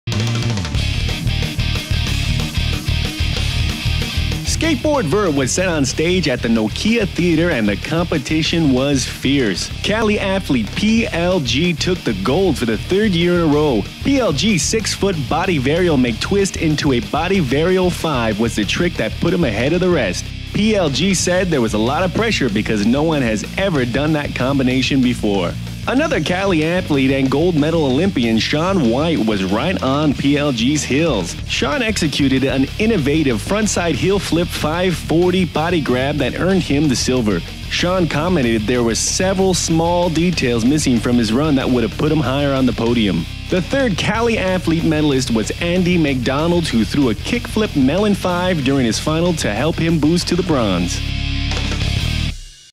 X Games 16 - Skateboard Vert Highlights
Enjoy Rally Car Racing highlights of this years events held at the LA Memorial Coliseum.

1. Pierre-Luc Gagnon  Carlsbad, Calif.     93.00
2. Shaun White          Carlsbad, Calif.     85.00
3. Andy Macdonald       San Diego, Calif.    79.00
4. Bucky Lasek          Baltimore, Md.       60.00
5. Sandro Dias          Santo Andre, Brazil  60.00

Photos by Max Simbron
Produced | Video | VO | GFXs | Edit by Timothy Adam
PA by Geo Lavrada

For LA36 broadcast
*All footage is practice